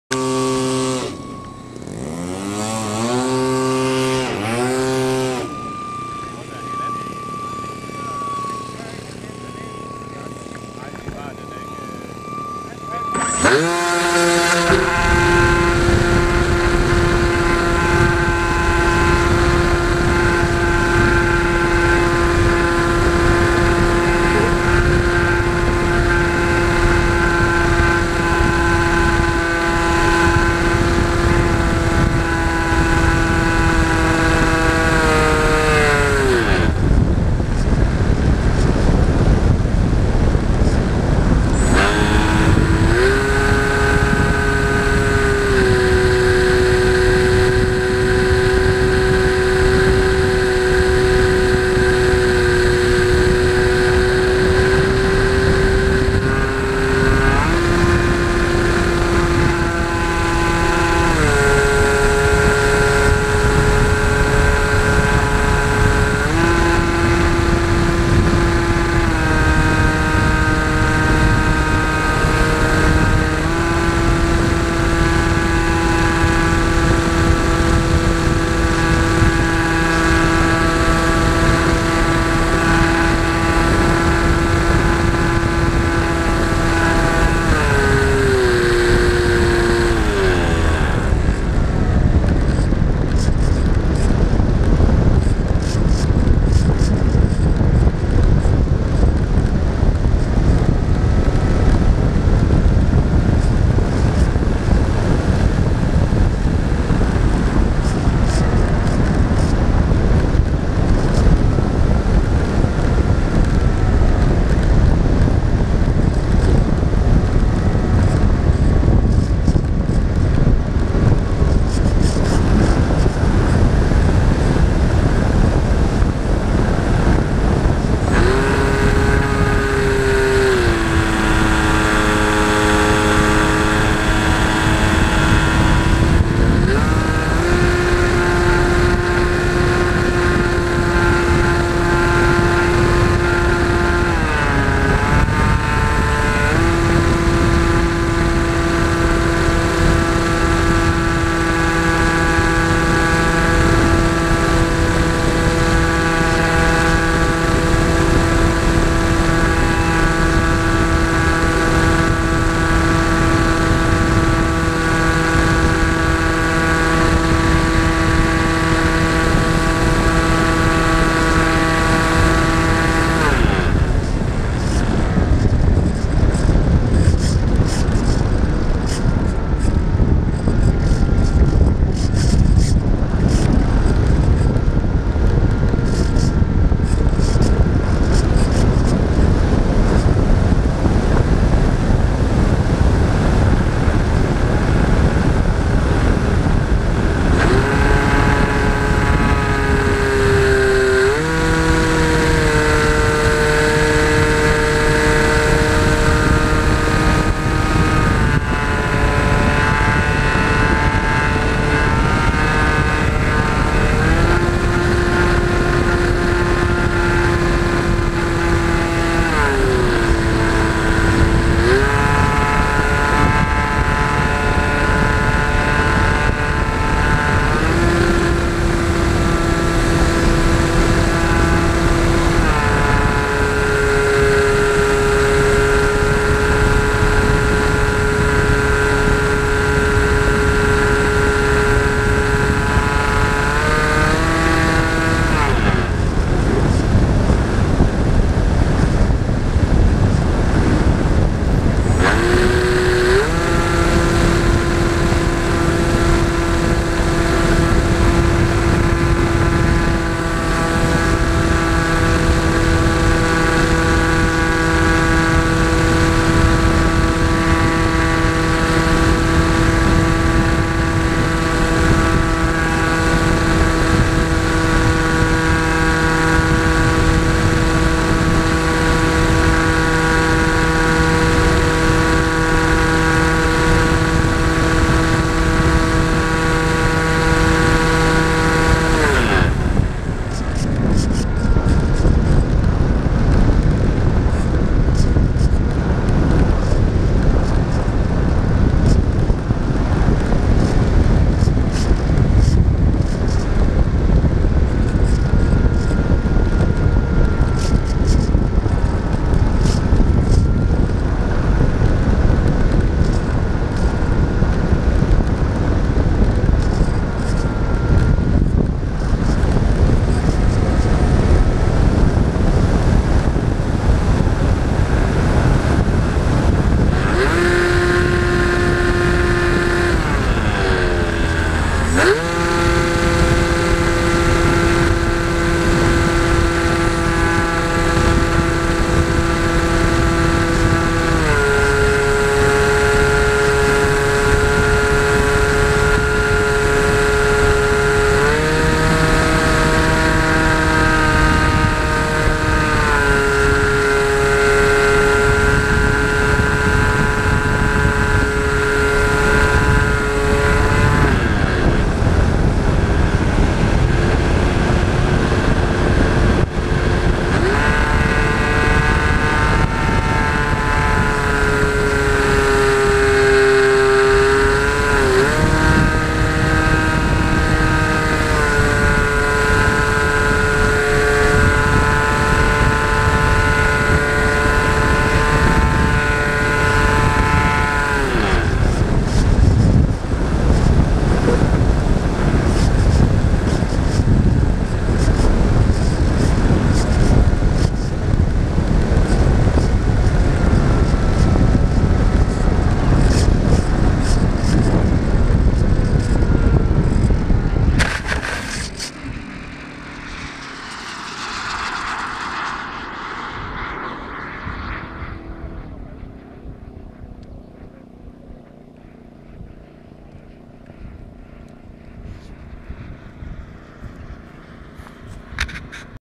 Brønderslev Modelflyveklub Marts 2014.
En fantastisk god start på sæsonen 2014 med godt vejr og massere af flyvning. Brønderslev Modelflyveklub - det er her det sker !